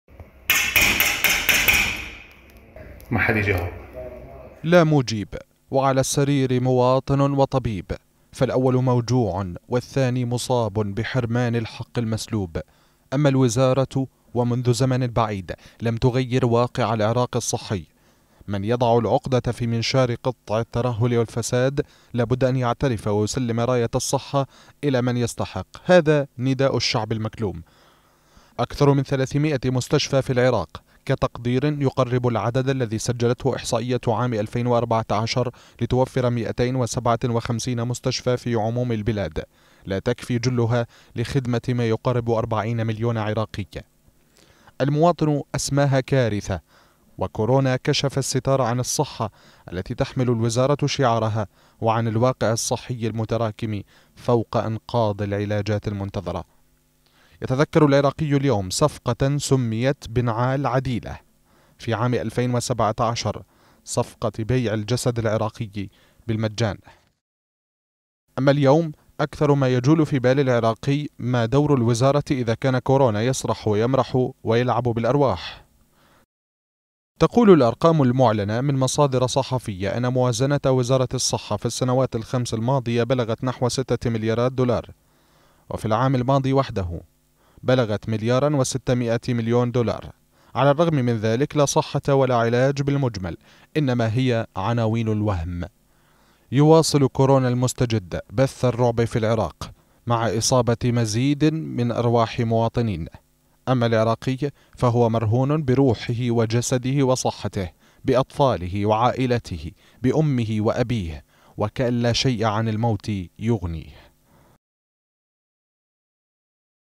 "نعال عديلة" تأكل أرواح العراقيين مجددا على شكل كورونا | تقرير
لا يعرف كورونا صغيرا ولا كبيرا، إنما هو القاتل الذي يدخل كل جسد لا حامي له أو معين، والمتابع اليوم يتساءل هل تقوم وزارة الصحة بواجبها الصحيح، أم أن كورونا وما سبقه من صفقات فساد سيدفع كلفتها العراقي بصحته وحياته..